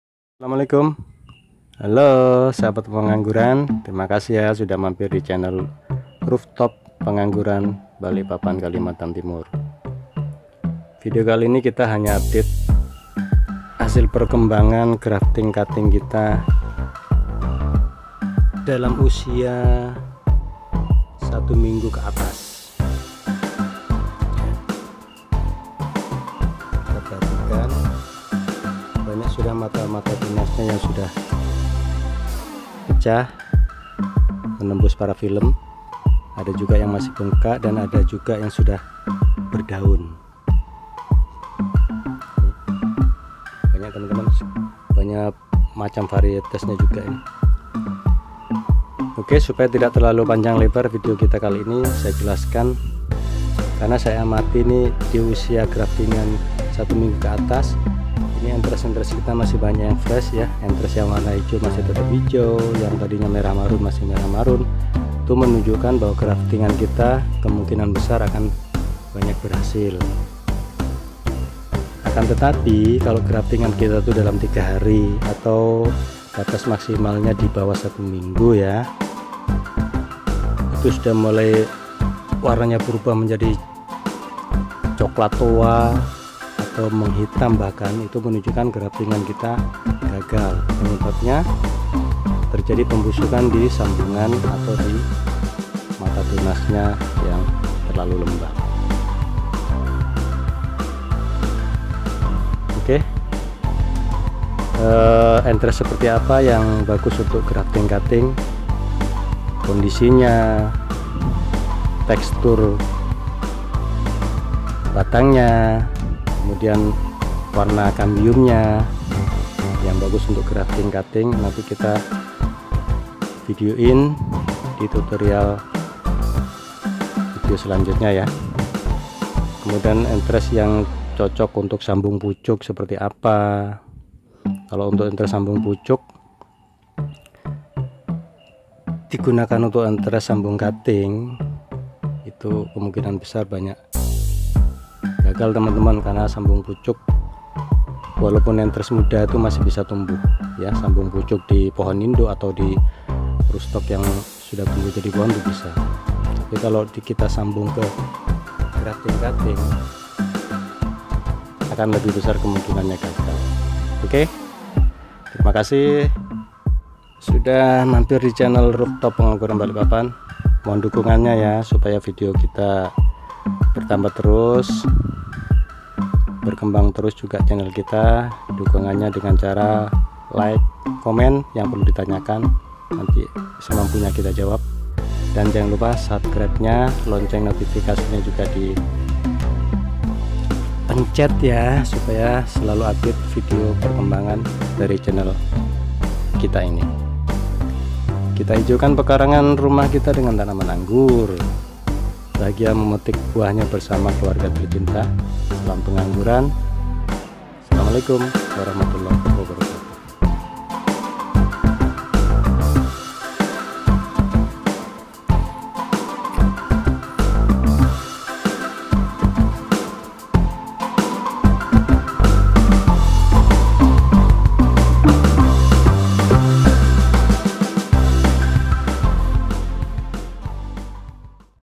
Proses Pembibitan Anggur | Rooftop Pengangguran Balikpapan | Bibit Anggur Balikpapan
Sekilas tentang Proses Pembibitan Anggur di Rooftop Pengangguran Balikpapan